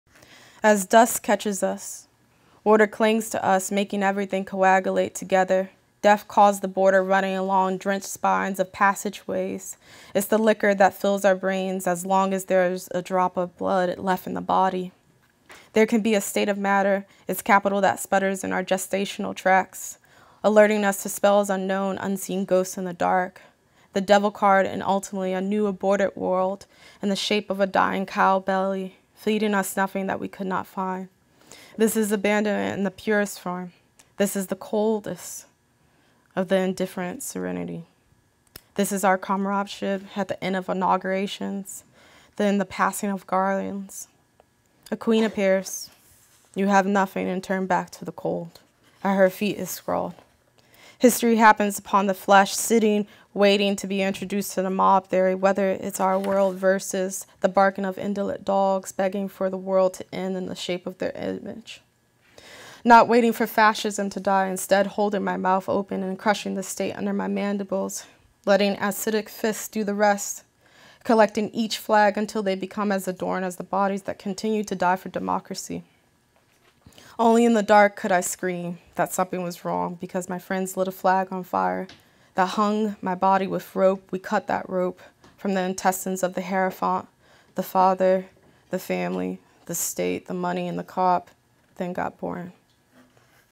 Jasmine Gibson — The Poetry Center
Jasmine Gibson reads "Ocean" from Don't Let Them See Me Like This (Nightboat Books, 2018), on February 8, 2019, at Moe's Books in Berkeley. The full program, presented as the second event of a double program in The Poetry Center's In Common Writers Series, features the complete reading by Gibson, followed by a reading by Juliana Spahr. Full program at Poetry Center Digital Archive